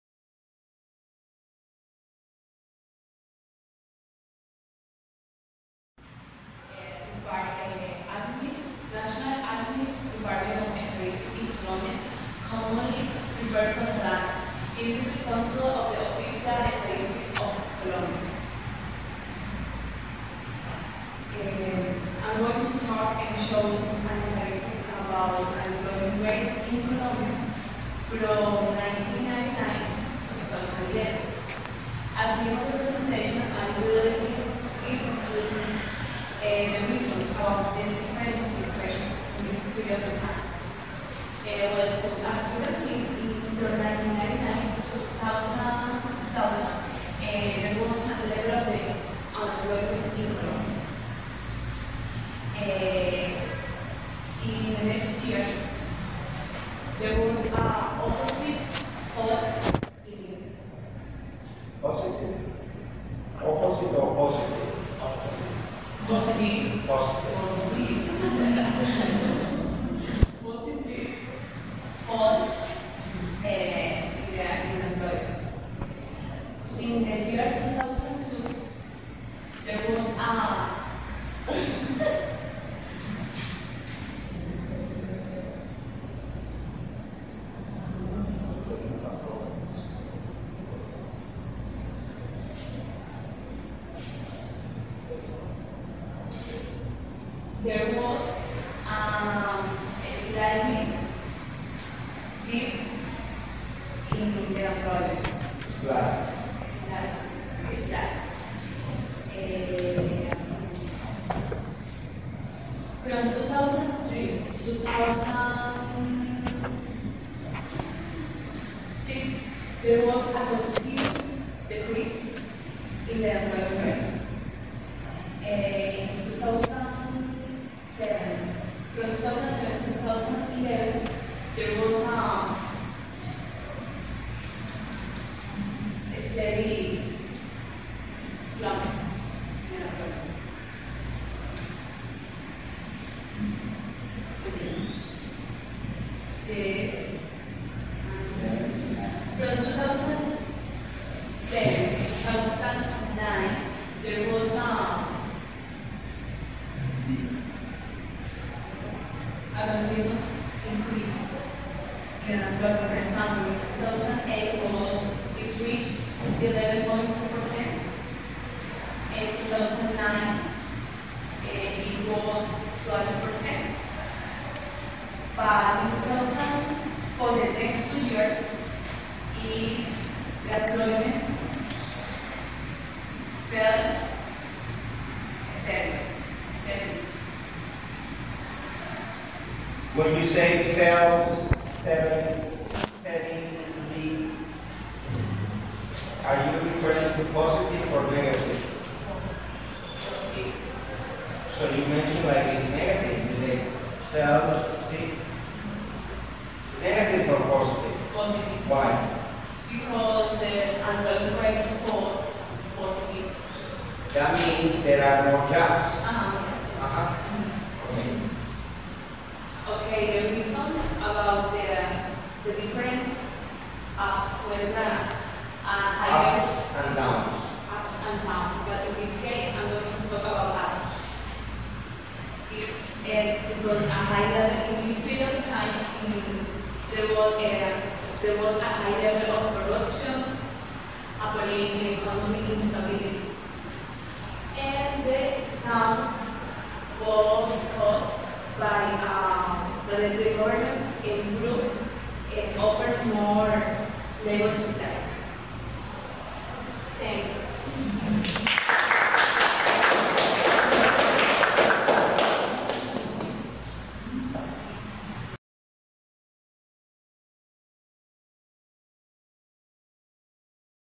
Adriana Lemus on Colombia's Unemployment Rate
Ruben munoz fifth semester english class featuring Adriana Lemus on Unemployment.
she did an outstanding job. well done.